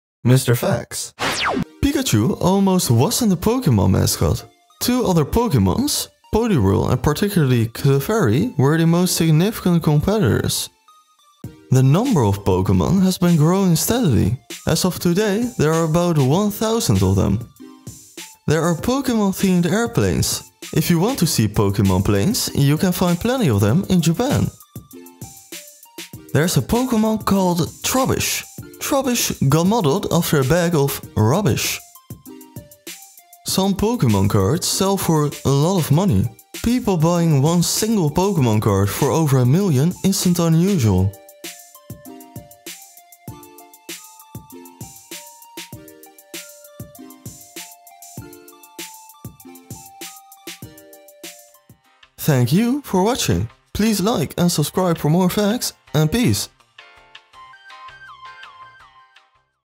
5 Facts about Pokémon
5 Facts about Pokémon. Did you know there are Pokémon themed planes?? Japan is where you might see some flying Pokémons. I hope you enjoy these five facts about Pokémon!

⏱️TIMESTAMPS⏱️
0:00 Intro
0:01 Fact one
0:21 Fact two
0:26 Fact three
0:36 Fact four
0:54 Fact Five

5 Facts about Pokémon

Pokémon cards fact 5 by Jarek Tuszyński from Wikimedia Commons